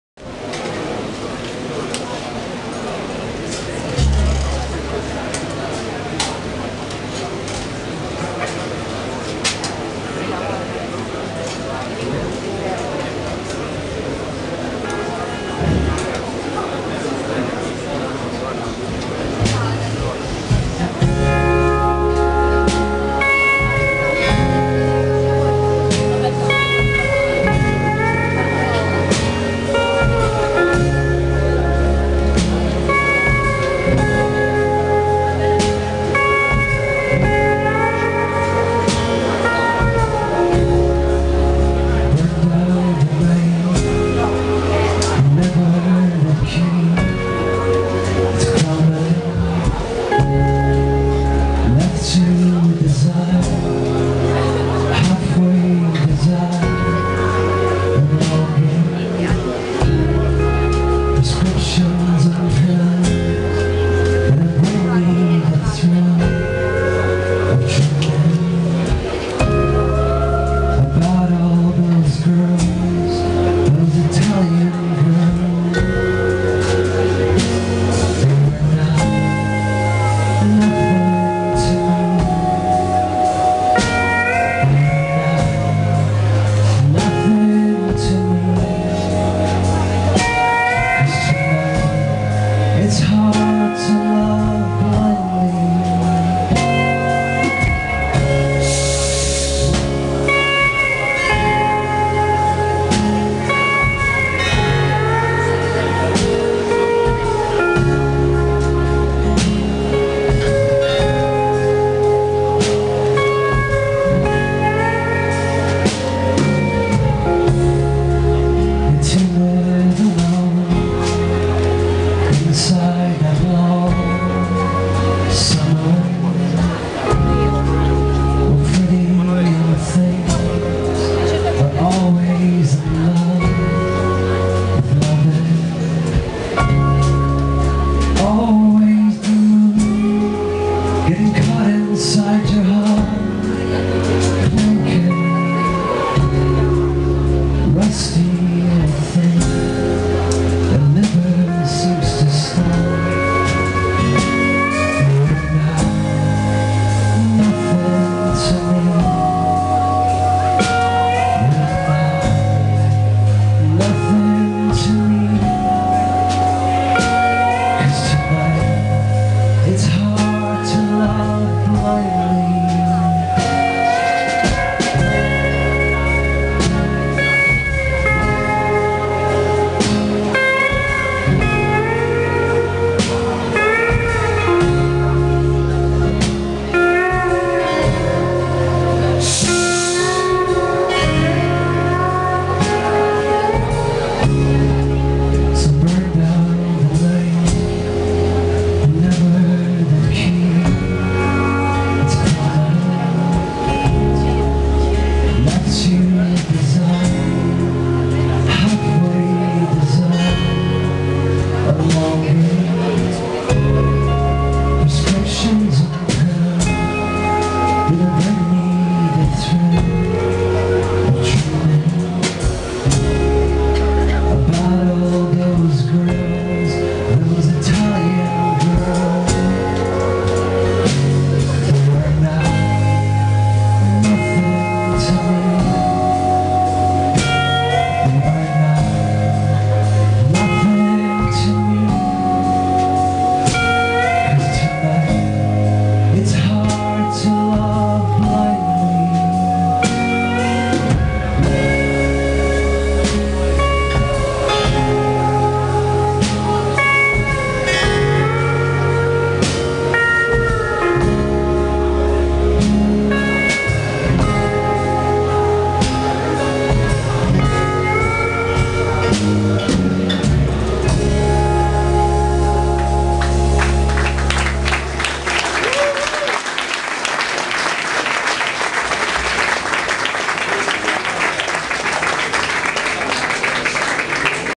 Barzin - unrleased? | Live 04 maggio 2010 @ Init, Roma, Italy 2 di 2
Barzin and live band, - truly hypnotic. I cant name the song, sorry, - I guess it's a new one. And my apologies for my crappy audio device.
On stage the same night: Efterklang + Heather Woods Broderick.